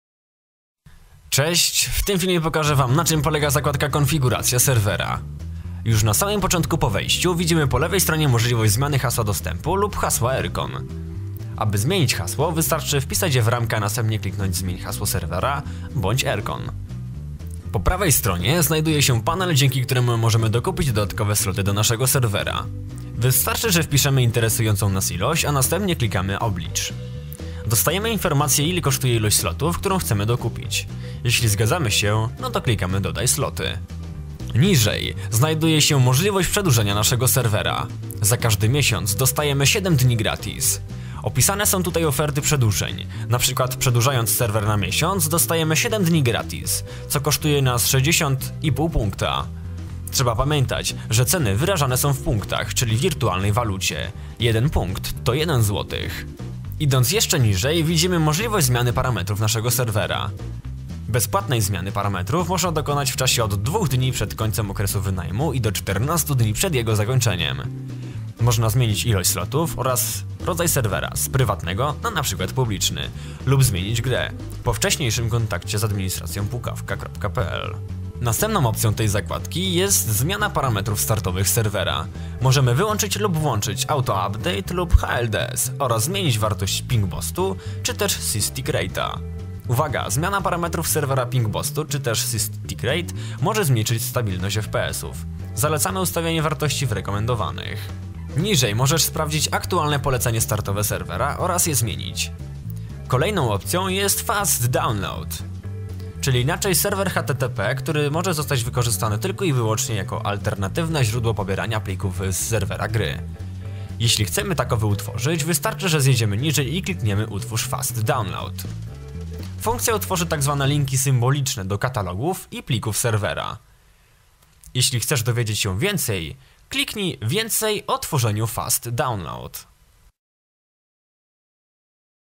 Panel Pukawka.pl - Counter-Strike 1.6 - Konfiguracja serwera
Poradnik korzystania z panelu administracyjnego Pukawka.pl